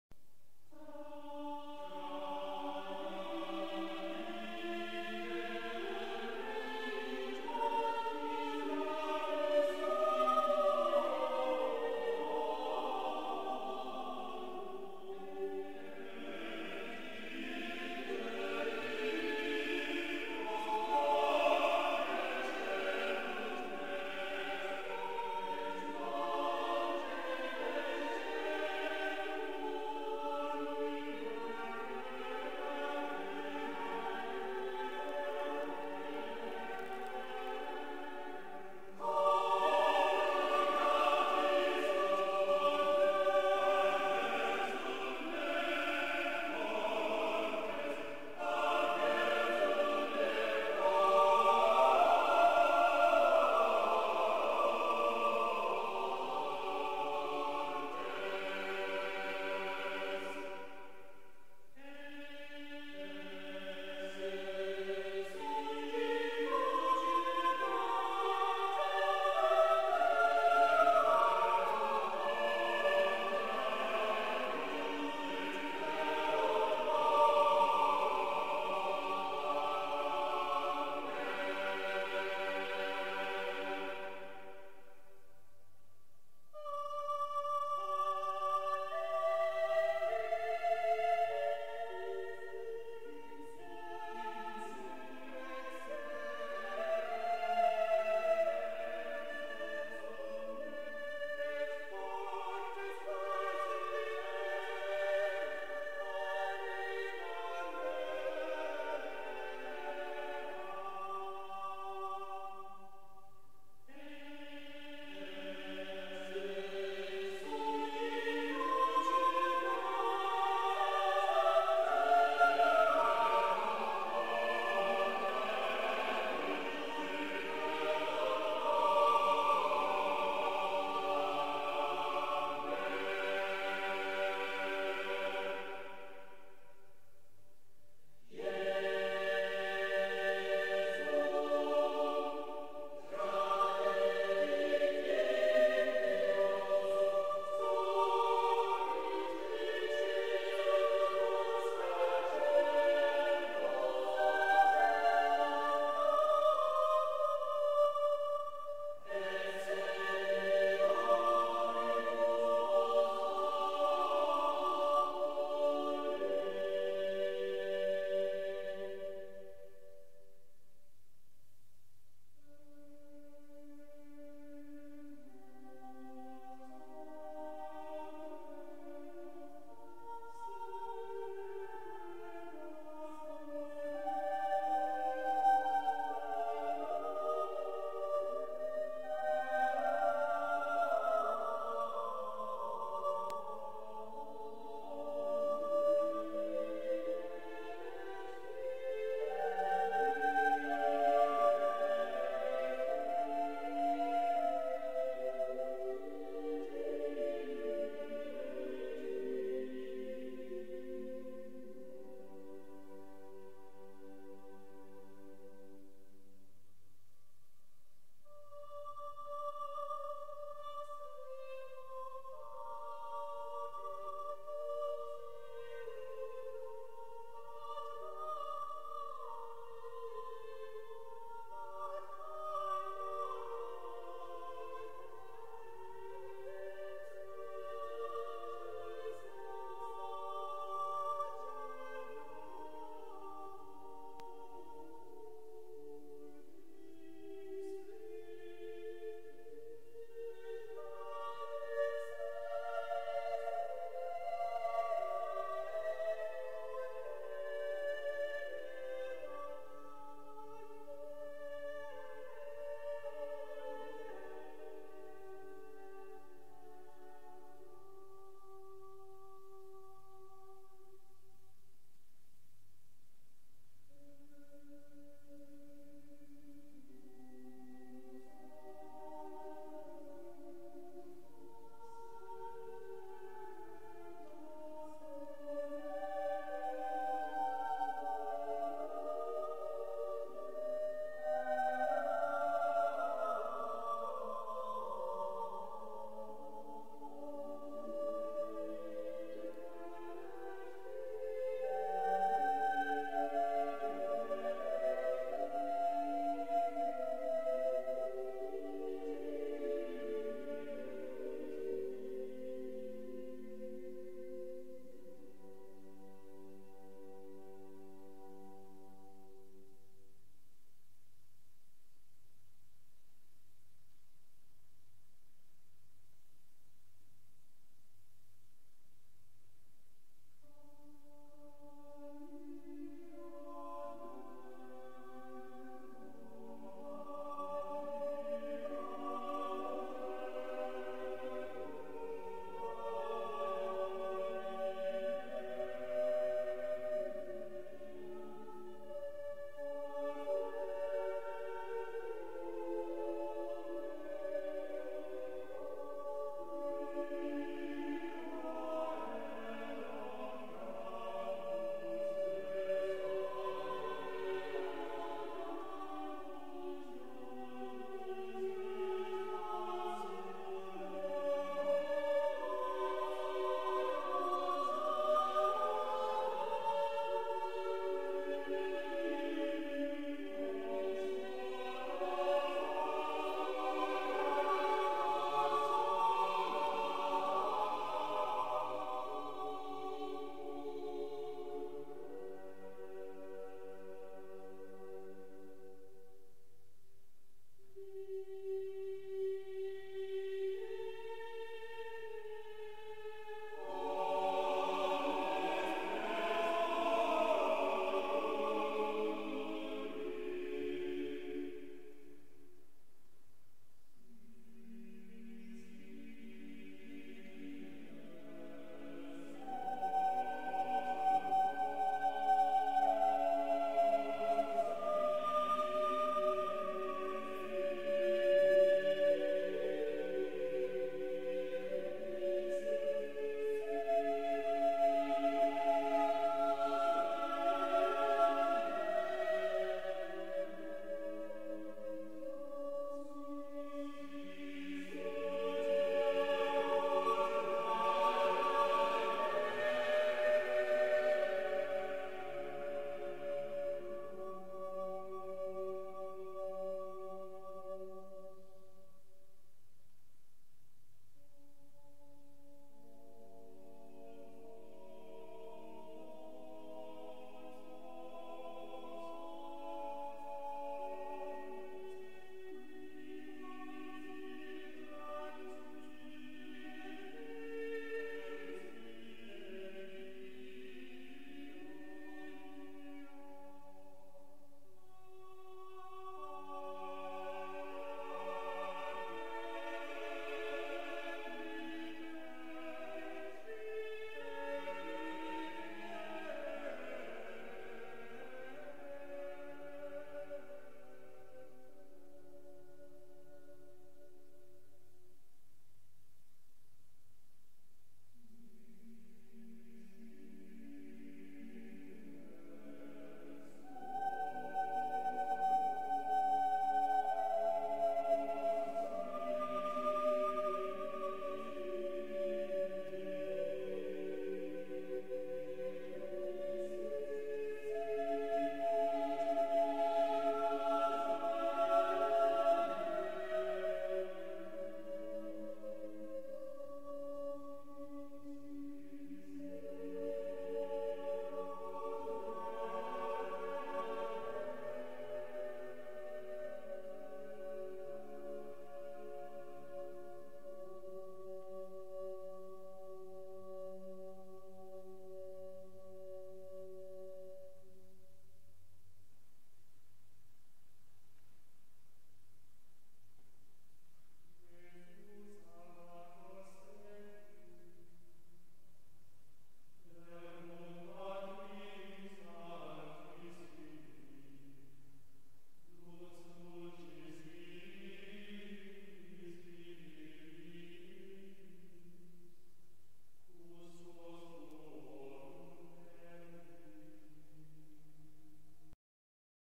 Frenze Church
Redundant now, this unspoilt little St. Andrew's is well worth seeking out.